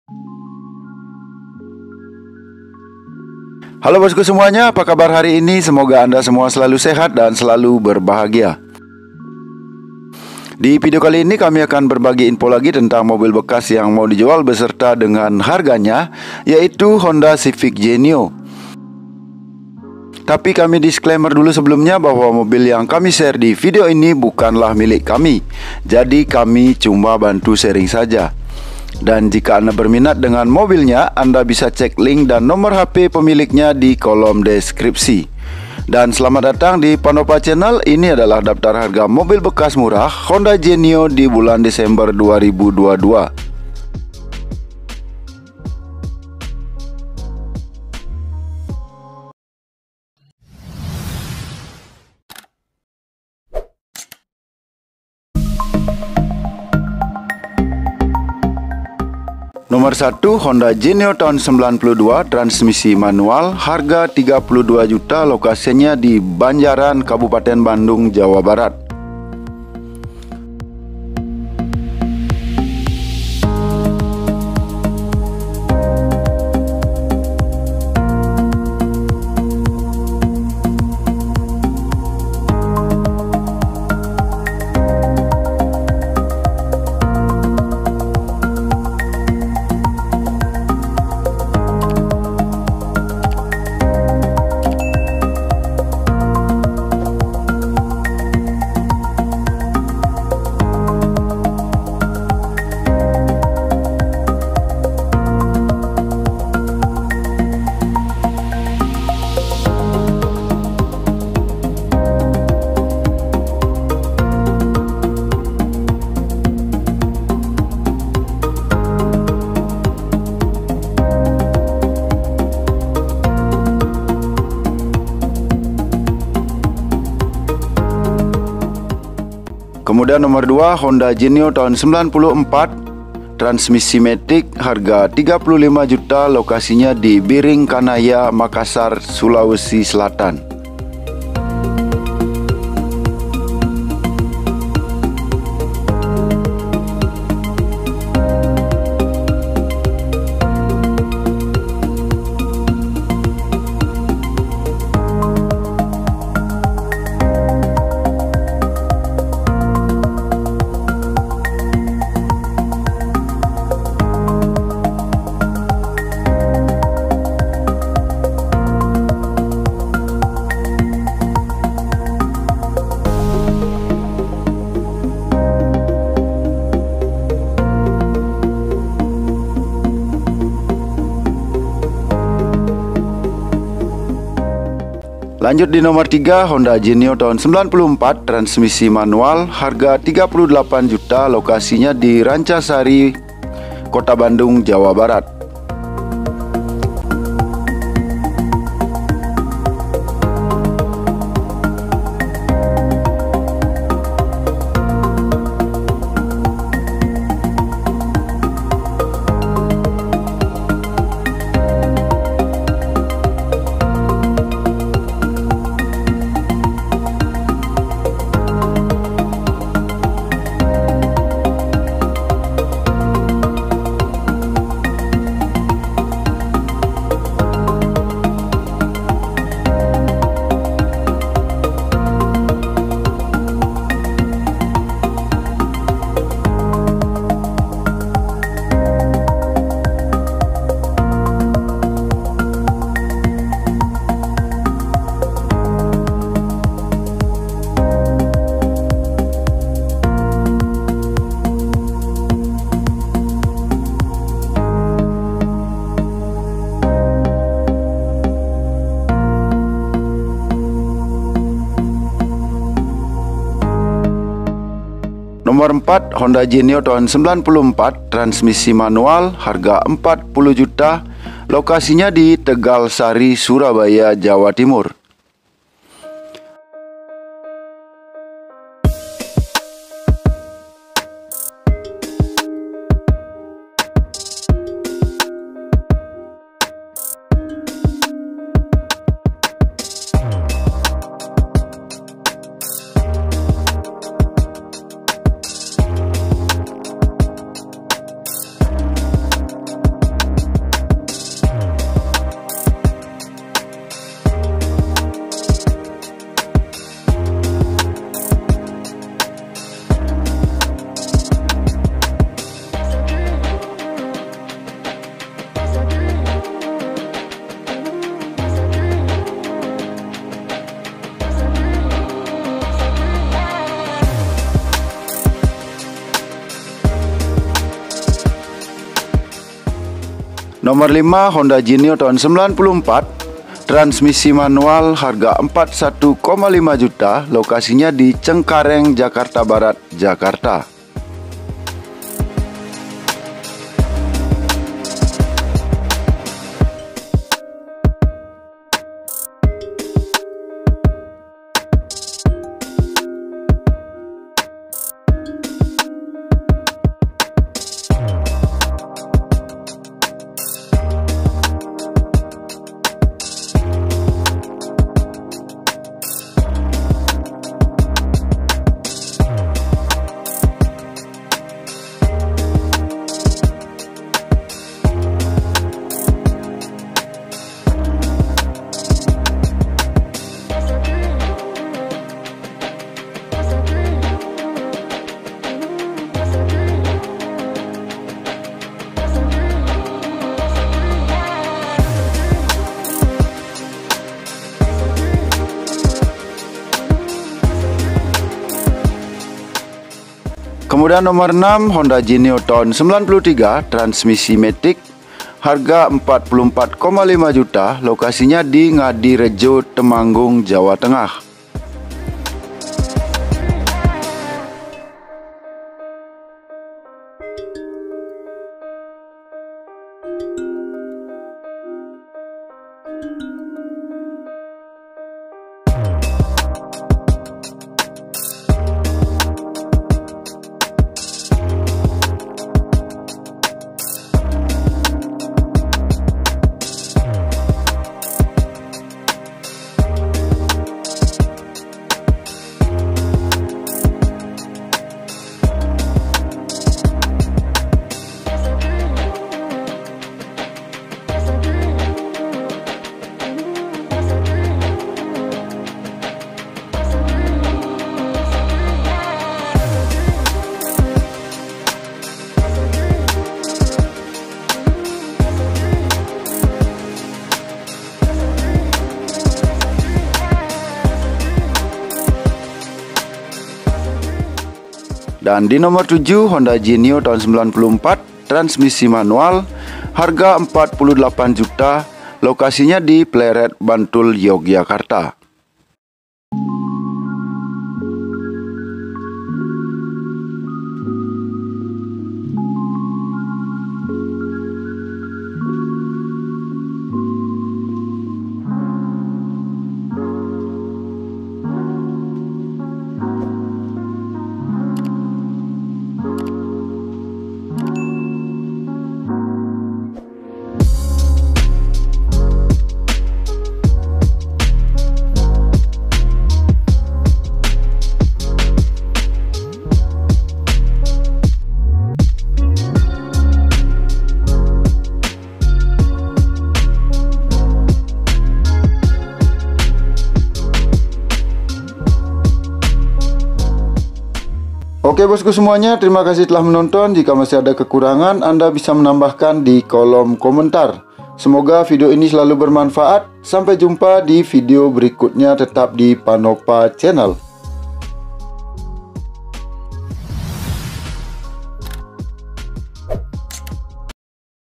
Harga mobil bekas Honda Civic Genio, di bulan Desember tahun 2022
Daftar harga mobil bekas Honda Civic Genio, di bulan Desember tahun 2022
Jika anda berminat dengan mobilnya, anda bisa cek link dan no hp penjualnya di kolom deskripsi
Link dan nomor HP penjualnya, kami cantumkan dibawah ini

Catatan : jika linknya sudah tidak ada, berarti mobilnya sudah laku 🙏
Berikut adalah link dan nomor HP penjualnya:

1. Honda Genio (1992)
1.6 BENSIN-MANUAL
Harga :  32 juta
Lokasi : Banjaran, Bandung Kab., Jawa Barat

2. Honda Genio (1994)
1.6 BENSIN-MATIC
Harga : 35 juta
Lokasi : Biring Kanaya, Makassar Kota, Sulawesi Selatan

3. Honda Genio (1994)
1.6 BENSIN-MANUAL
Harga : 38 juta
Lokasi : Rancasari, Bandung Kota, Jawa Barat

4. Honda Genio (1994)
1.6 BENSIN-MANUAL
Harga: 40 juta
Lokasi: Tegalsari, Surabaya Kota, Jawa Timur

5. Honda Genio (1994)
1.6 BENSIN-MANUAL
Harga: 41,5 juta
Lokasi : Cengkareng, Jakarta Barat, Jakarta D.K.I.

6. Honda Genio (1993)
1.6 BENSIN-MATIC
Harga :  44,5 juta
Lokasi : Ngadirejo, Temanggung, Jawa Tengah

7. Honda Genio (1994)
1.5 BENSIN-MANUAL
Harga : 48 juta
Lokasi : Pleret, Bantul Kab., Yogyakarta D.I.

Simak videonya sampai habis ya bosku 🙏